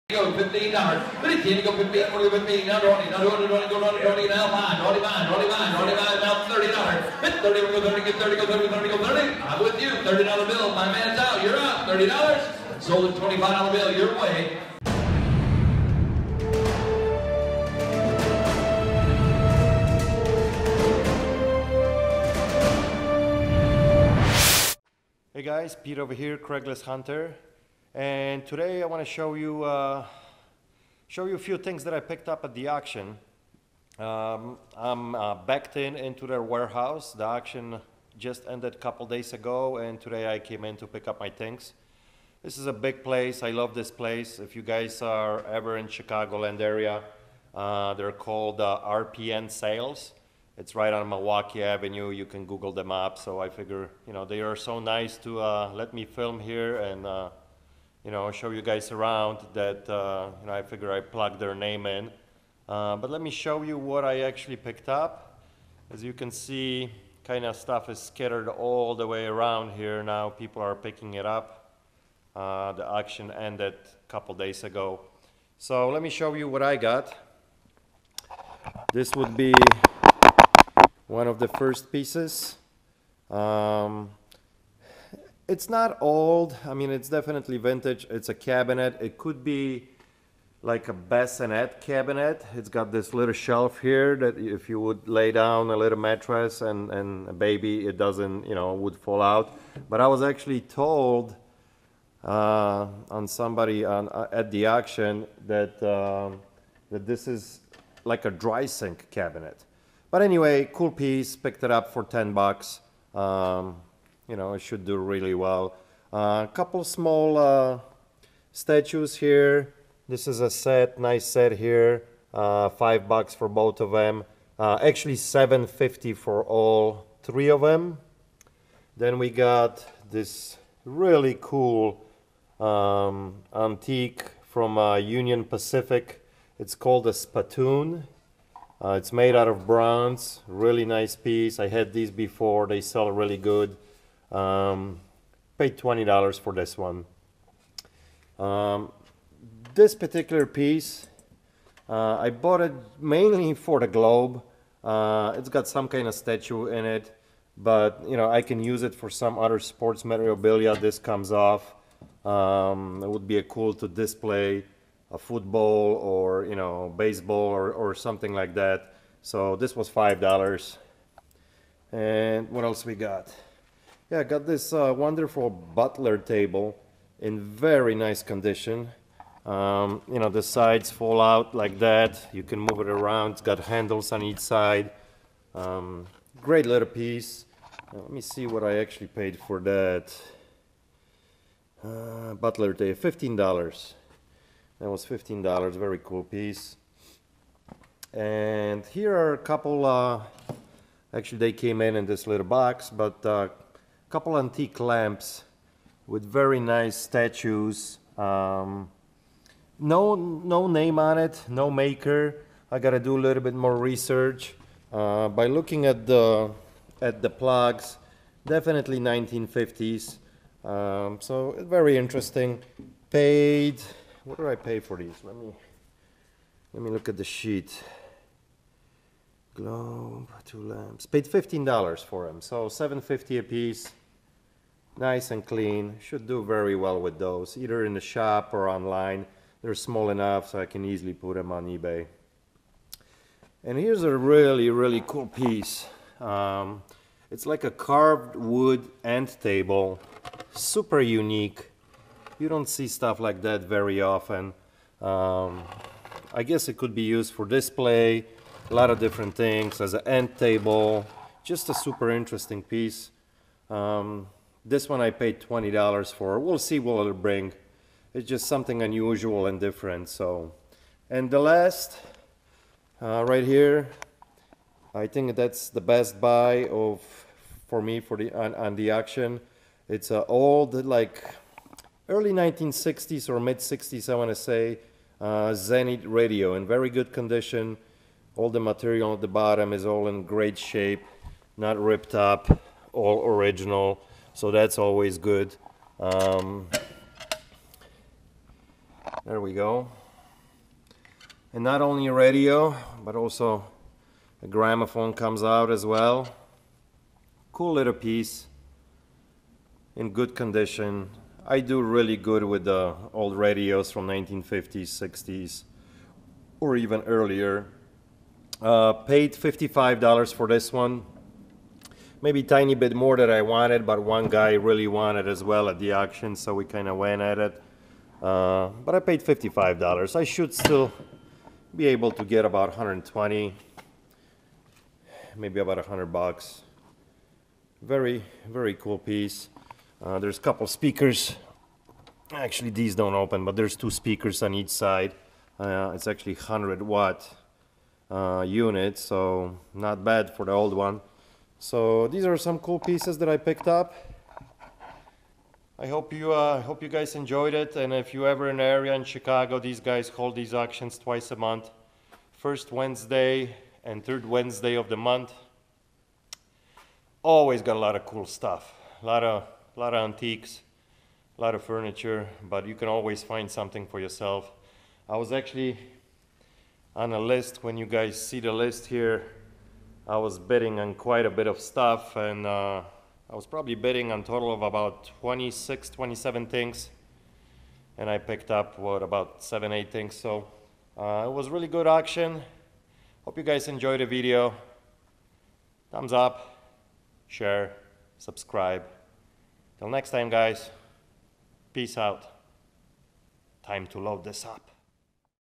►Things I scored at the Auction #37 Craigslist Hunter
Sourcing from your local auctions is a great way to pick-up lots of stuff very cheap. Let me show you what I got on my lest visit at the Auction House, Antique Zenith Radio.